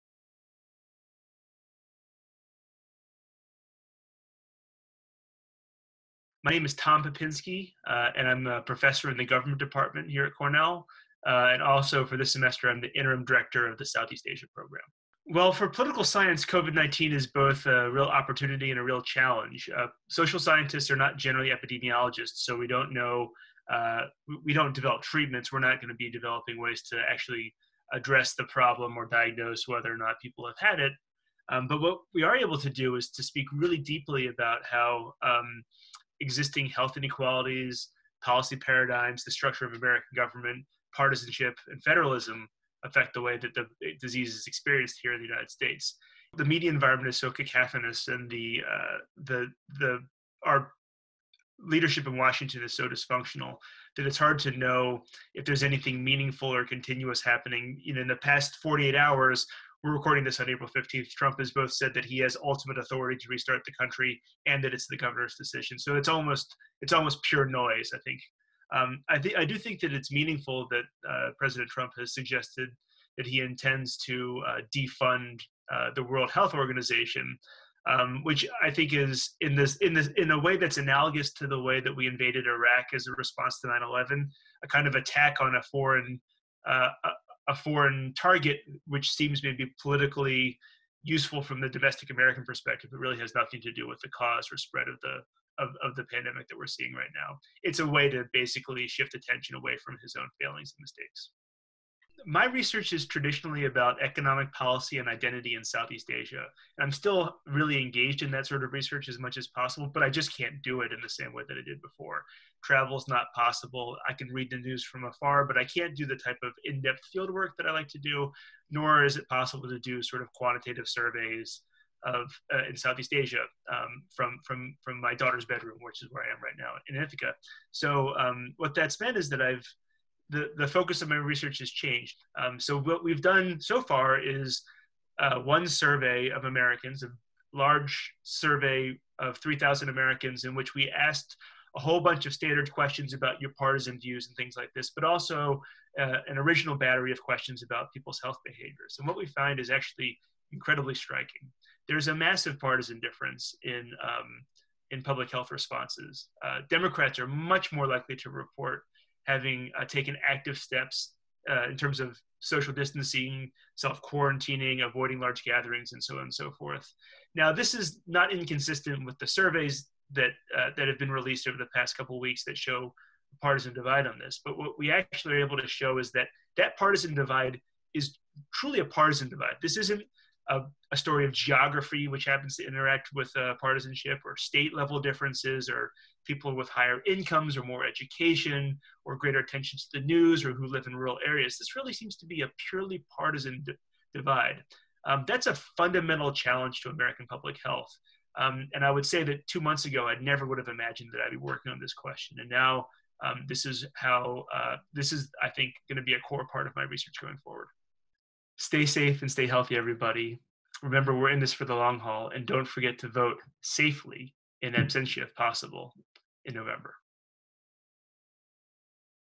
COVID-19 impact: Tom Pepinsky on the partisan divide
Thomas Pepinsky, an expert on economic policy at Cornell University, discusses President Donald Trump’s decision to halt funding to the World Health Organization. He also talks about his new research, which finds Democrats are much more likely to report taking active steps to combat the spread of COVID-19. This “massive partisan difference,” he says, poses “a fundamental challenge to American public health.” Pepinsky is a professor of government in the College of Arts and Sciences. He studies economic policy and identity in Southeast Asia.